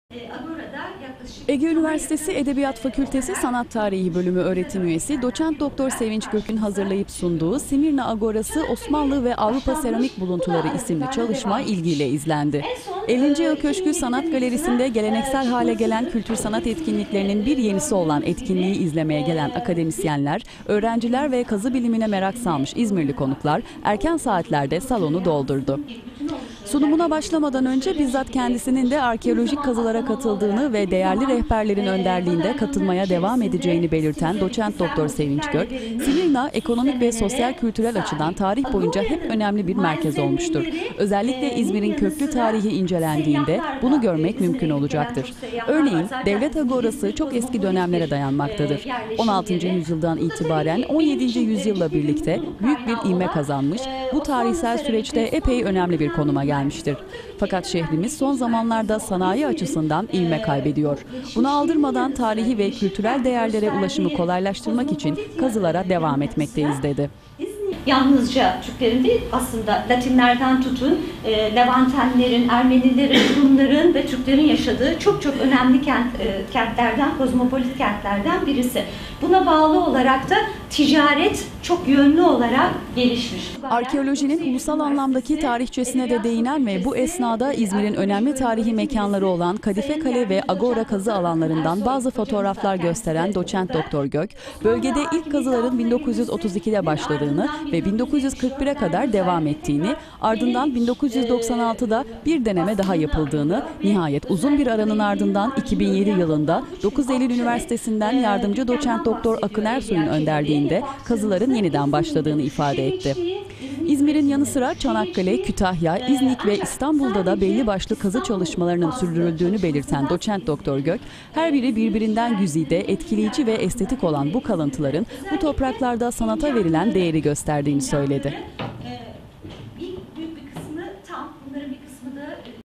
Ege'de Arkeoloji Paneli
Ege Üniversitesi Edebiyat Fakültesi Sanat Tarihi Bölümü Öğretim Üyesi Doç. Dr. Sevinç Gök'ün hazırlayıp sunduğu 'Smyrna Agorası Osmanlı ve Avrupa Seramik Buluntuları' adlı çalışma katılımcılar tarafından yoğun ilgi gördü. Doç. Dr. Gök, İzmir'in tarih boyunca önemini hiç yitirmediğini vurguladı.